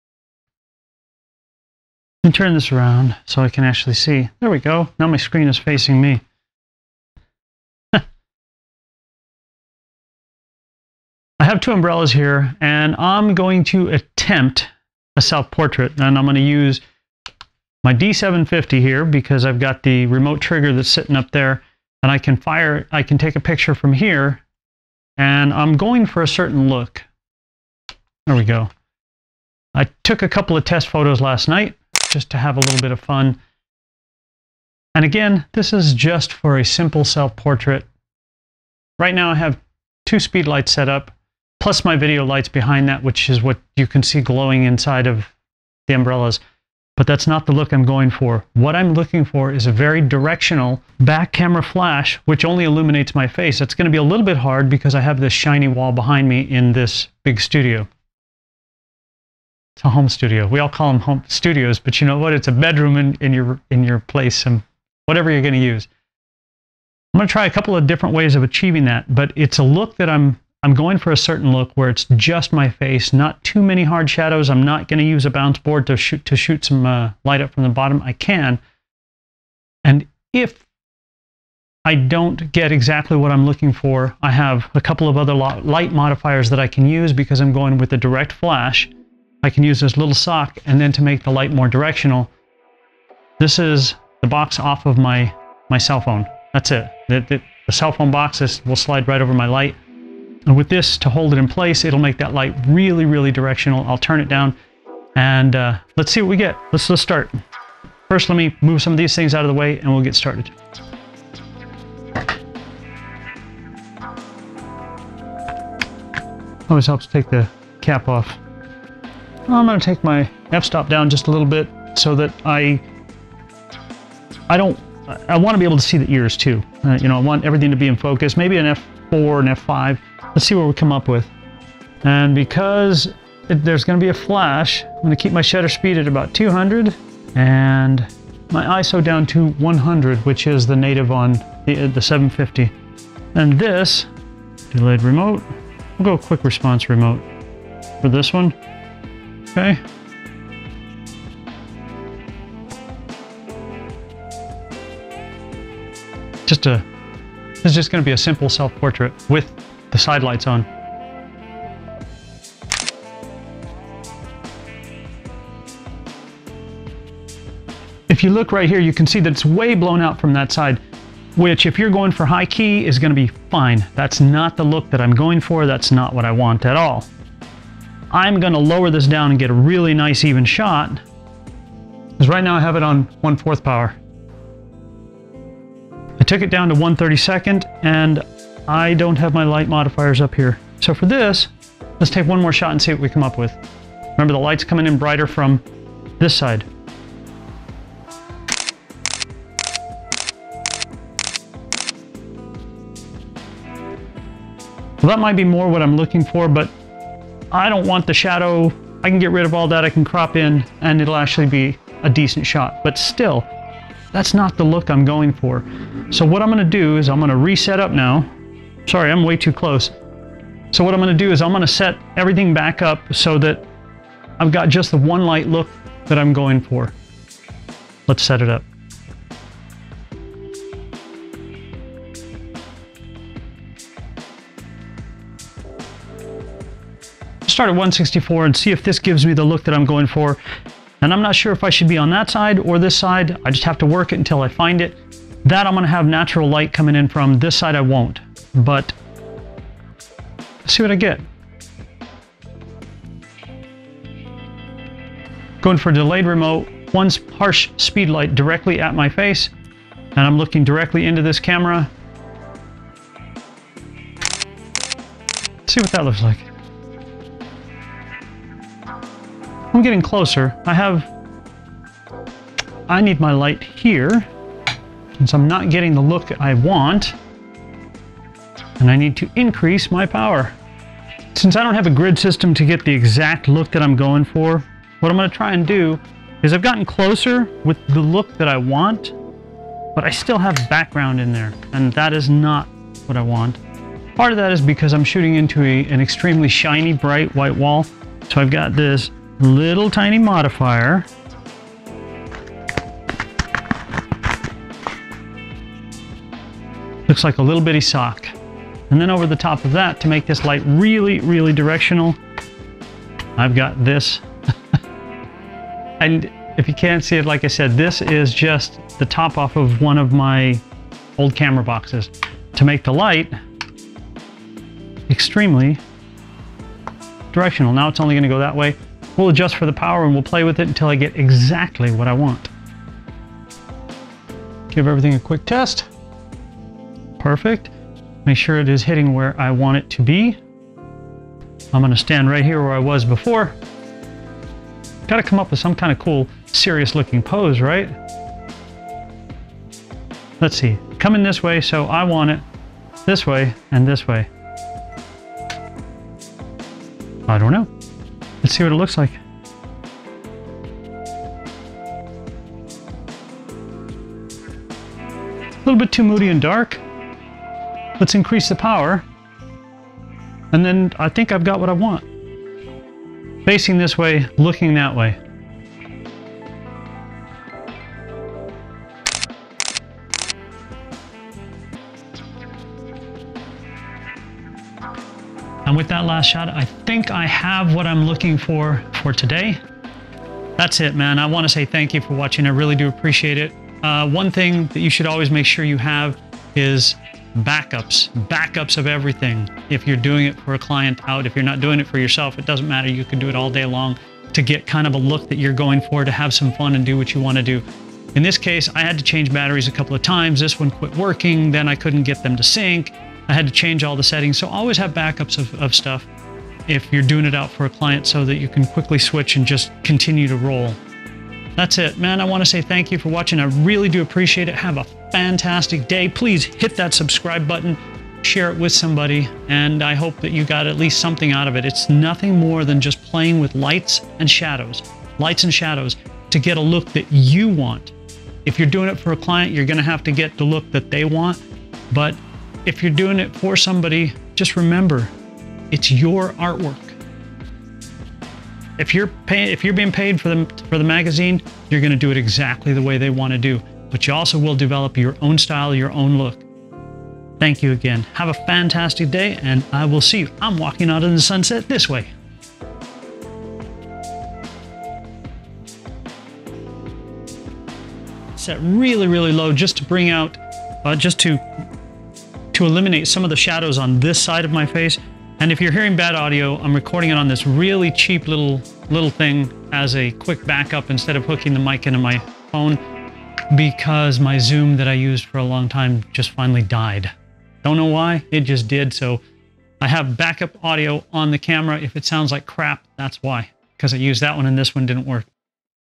Taking a self portrait with speed lights, getting a look I want. A selfie..
My portfolio for those needing an event photographer!

prior used lens tested.

Thank you so much you watching what I love to doing!

And yeah, Face Book too!

Shot with..
Nikon D750, Nikon 50mm f/1.8
Sony video camera.